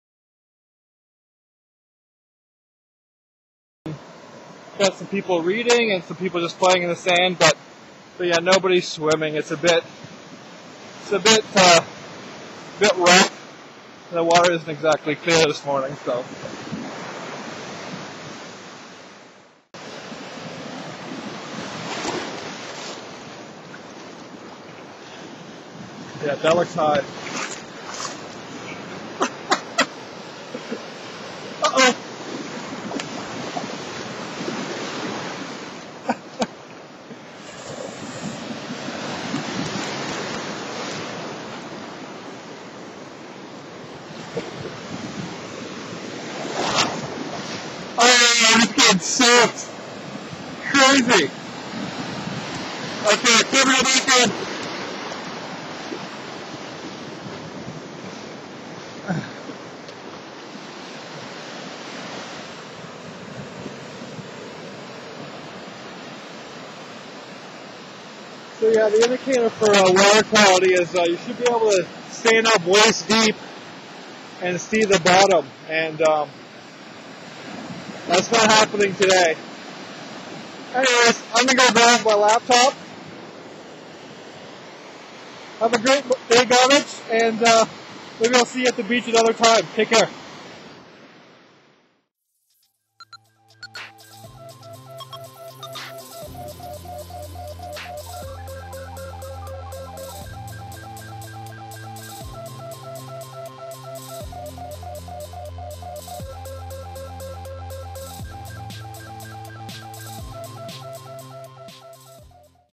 Good Morning Goderich Beach Report!
Good Morning Goderich Episode 21 - July 29  2010
Beach Report!

Was down at the beach this morning to see what was going on. Met Keisha from the Huron County Health Unit who was taking water samples to test the water quality - they have created a great website to let us know when and where it is good to swim.

"Don't swim in the water if you can't see your feet (at waist depth of an adult)."

Tried to show you that in practice, but the waves doused the laptop!